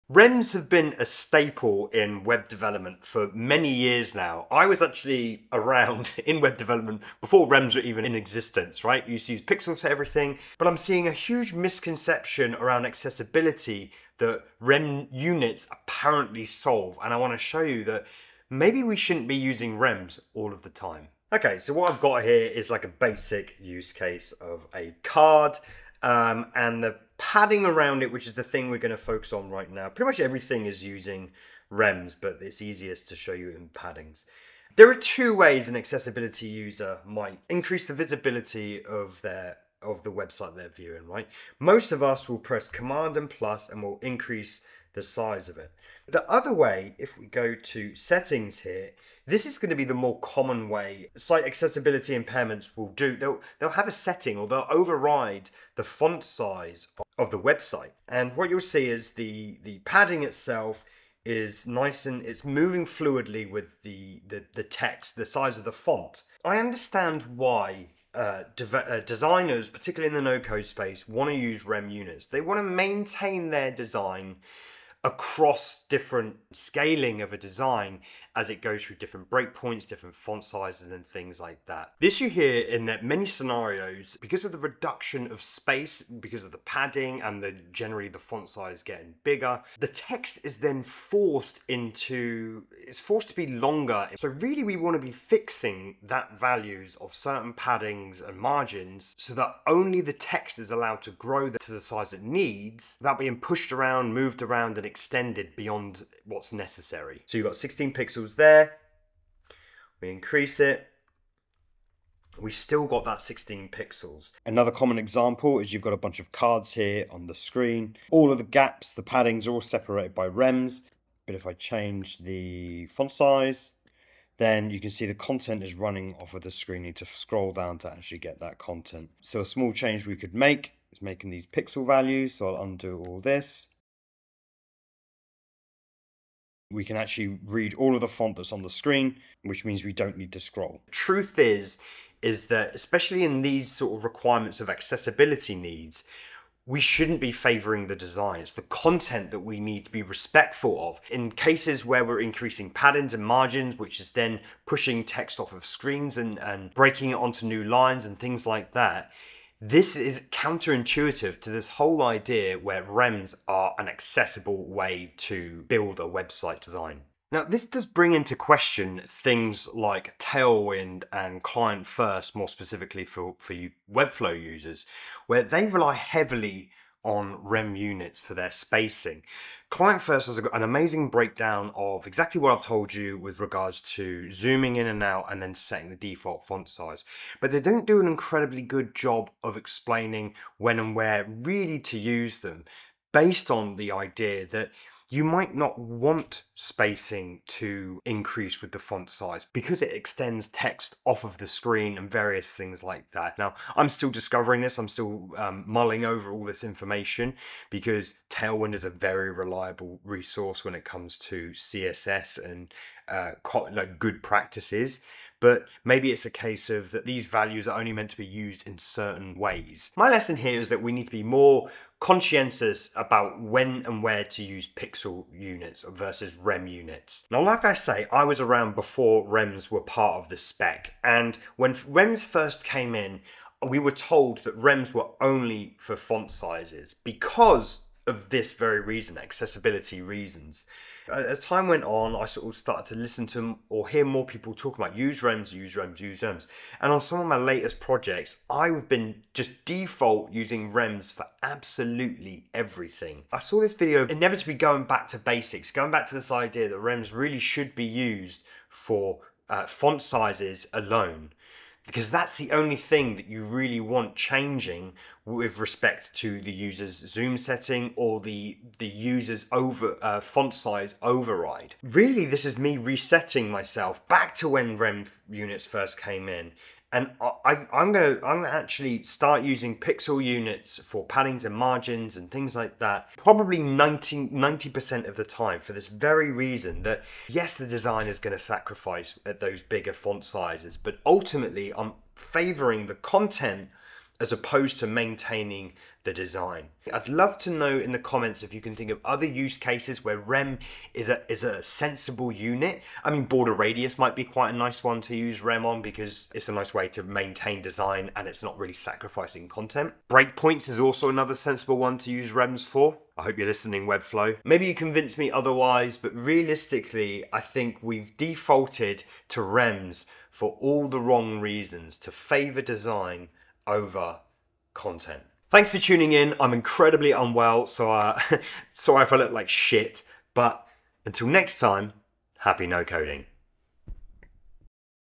No Coders Are Using REM Units WRONG
⏰ Timestamps
00:00 - Intro
00:22 - Using REM for padding/margin demo
02:10 - Changing to px example
02:22 - We shouldn't be favouring design for accessibly
02:47 - Tailwind and Client First
03:41 - Going back to basics

Sony A7c II
Sony 16-35mm
Deity V-Mic D4 Mini

iPad Pro M4
Apple Magic Trackpad